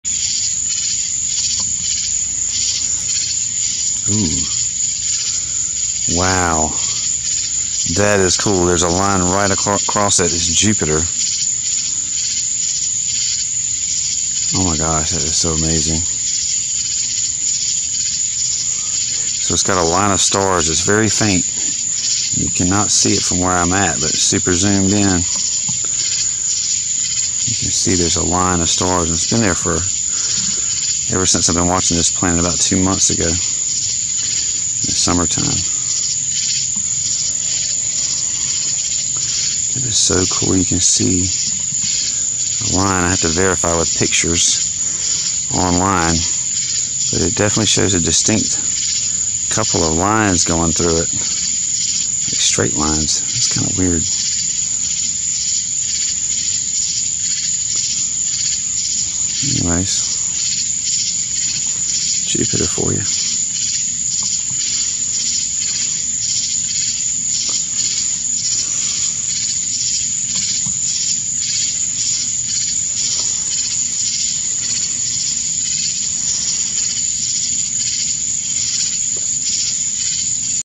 JUPITER SUPERZOOMED - REAL PLANET FOOTAGE, UP CLOSE / NIKON P900
Jupiter zoomed in with Nikon P900